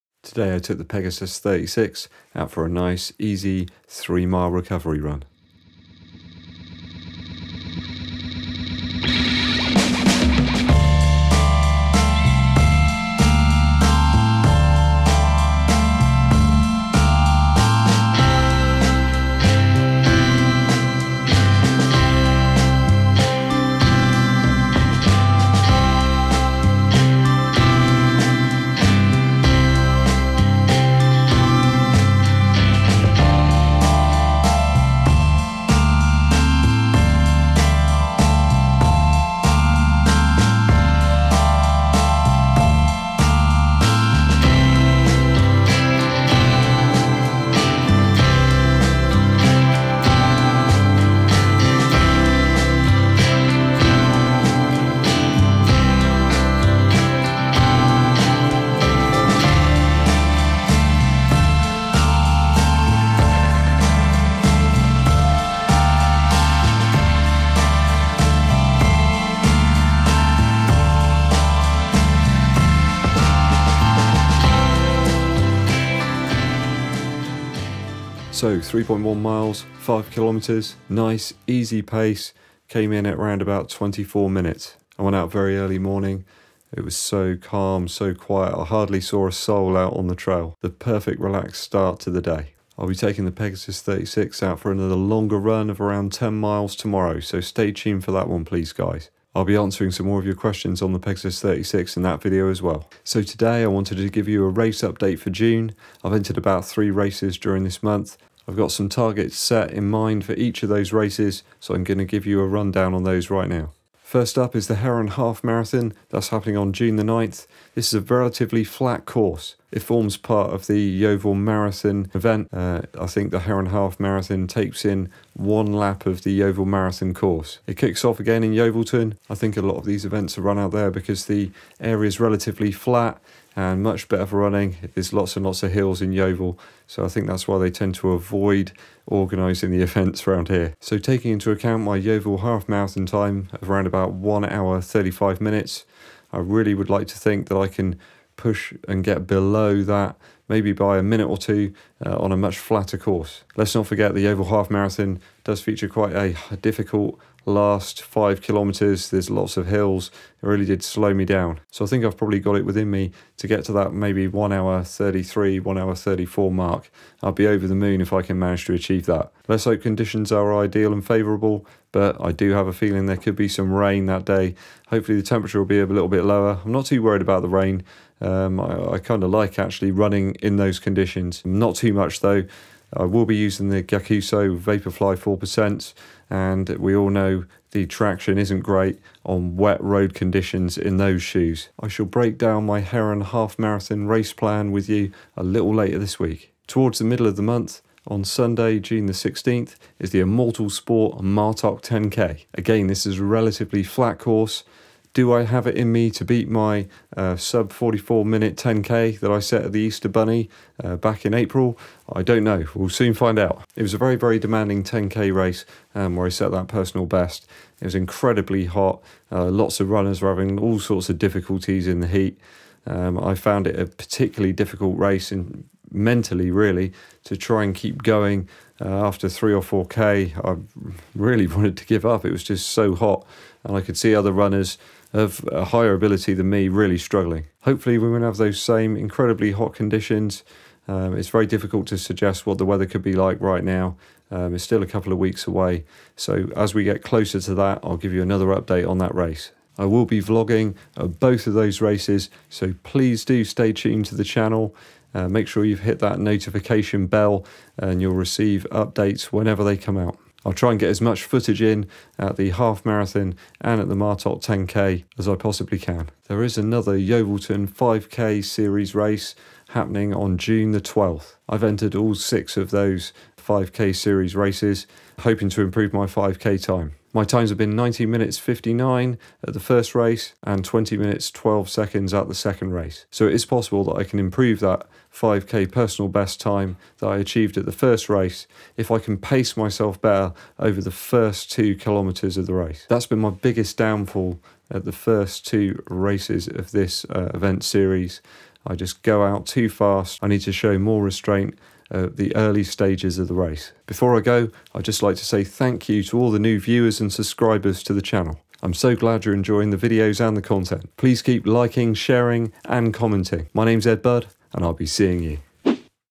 Easy miles in the Pegasus 36 and June Race Schedule
A short montage of footage from an early morning low mileage session in the Pegasus 36. I discuss the up coming races in my schedule and target times for each race along with my current personal bests for 5k,10k and the half marathon distance.